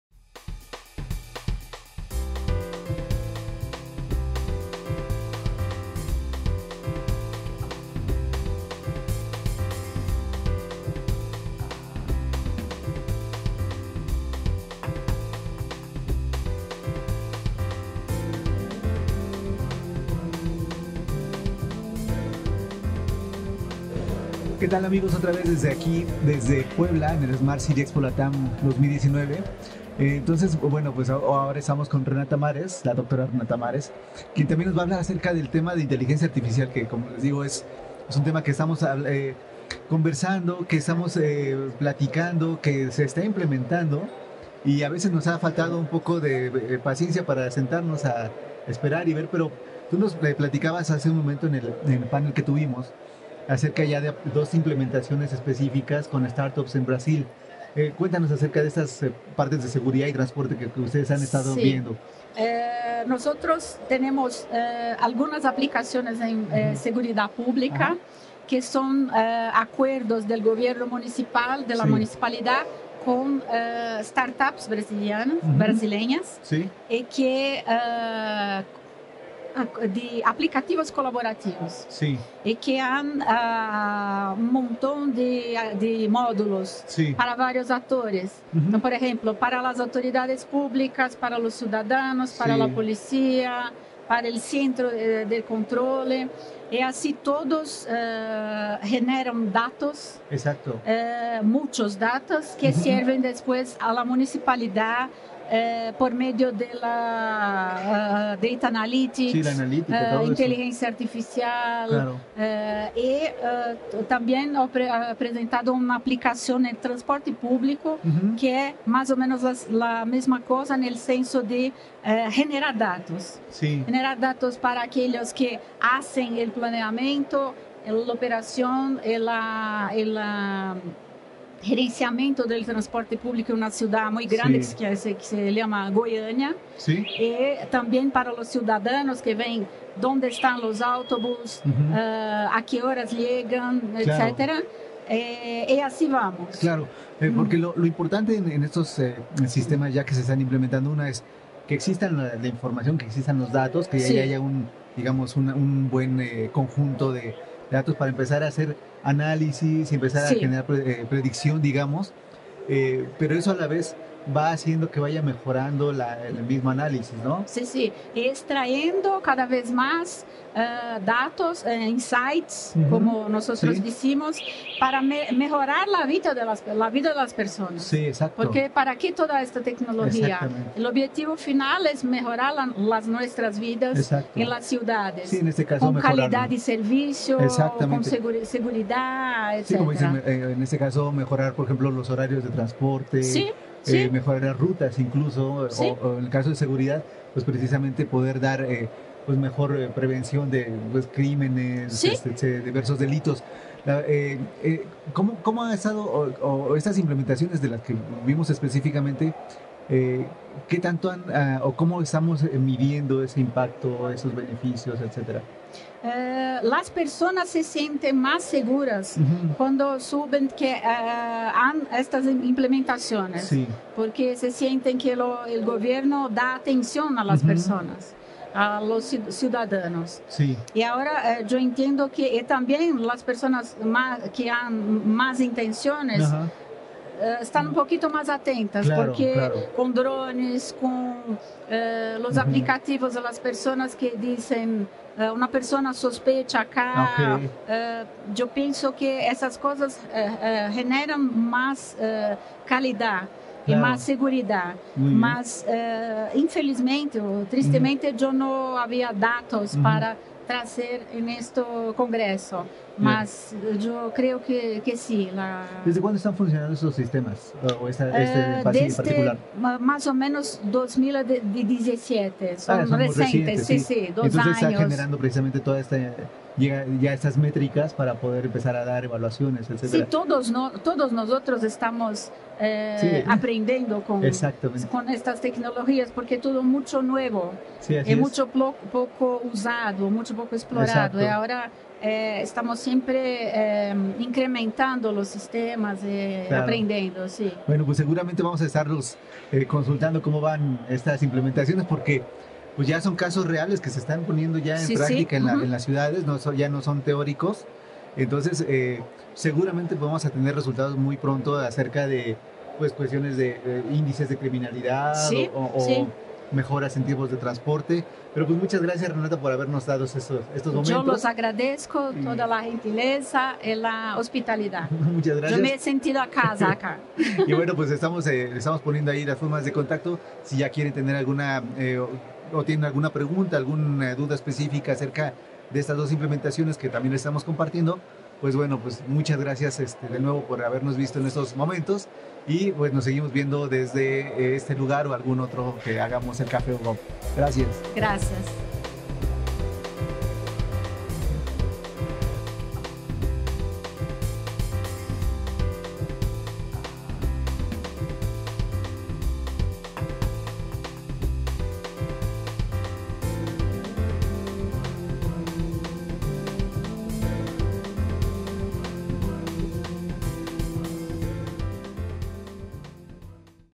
Ciudades inteligentes y acciones colaborativas en Brasil
Para mejorar la calidad vida y la seguridad de las personas en las ciudades, Renata Maré, Investigadora y Profesora de la Universidad de Sao Paulo, Brasil (en el núcleo de investigación USP Ciudades), nos habla de dos nuevas implementaciones brasileñas de Inteligencia Artificial y Analítica en las que gobierno municipal y start ups colaboran estrechamente.